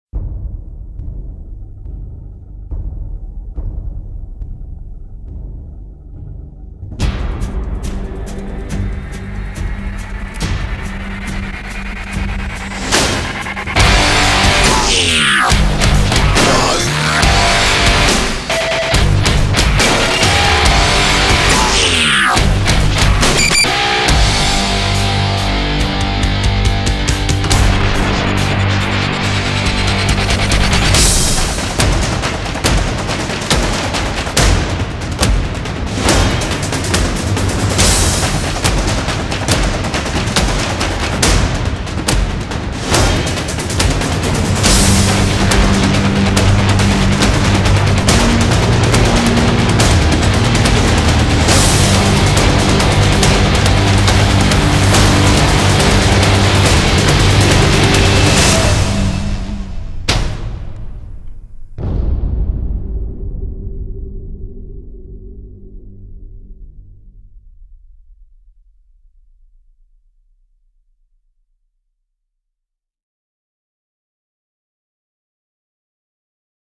Amphibious Zoo Sports Reel - music for sports production
Highlighting productions using Amphibious Zoo's music library and custom music services.